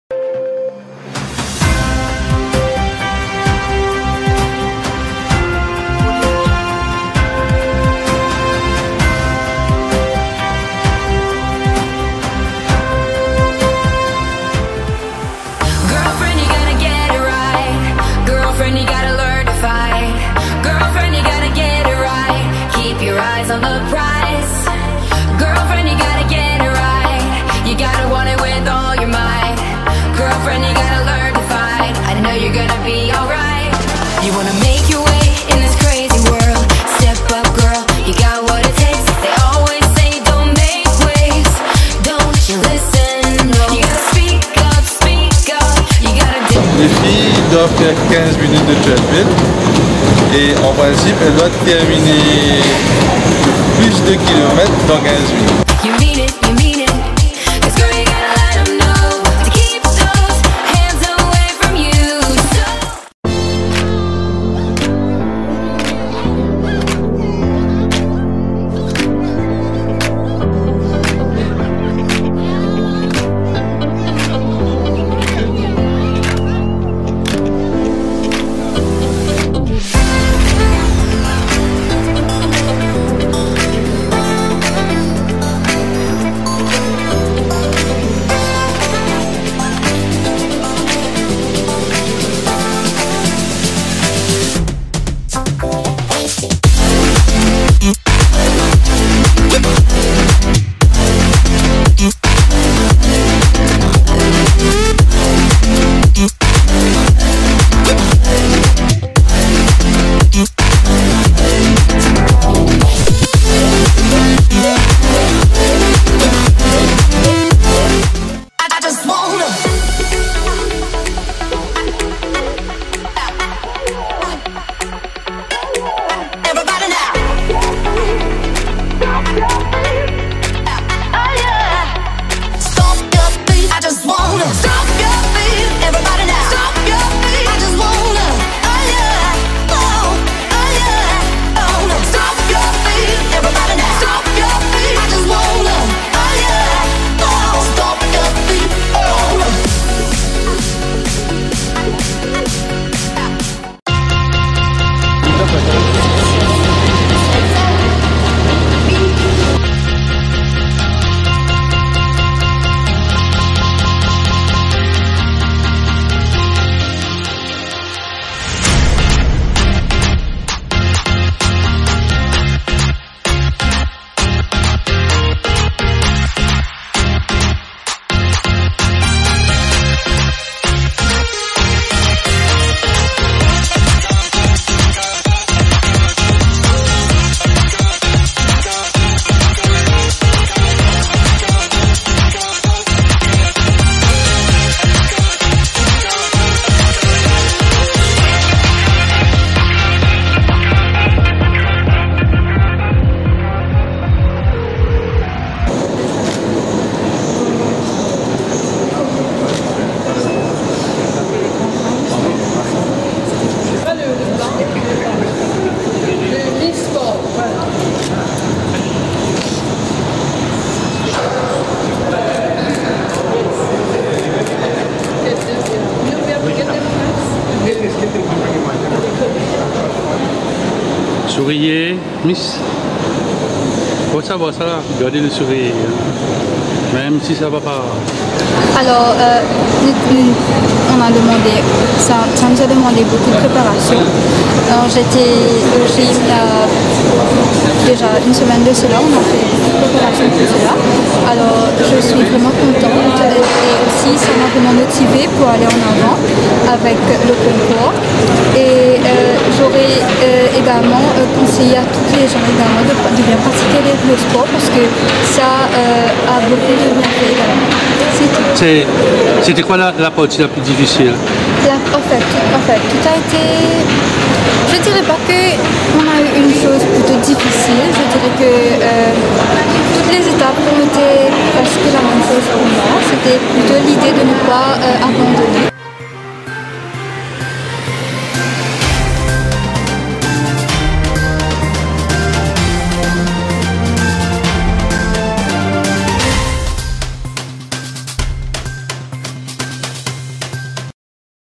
Maurice Info - [Exclusivité] Sheekha Seebaluck est la Miss Sport de Miss Mauritius 2018
Une actualité par heure - Maurice Info - [Exclusivité] Sheekha Seebaluck est la Miss Sport de Miss Mauritius 2018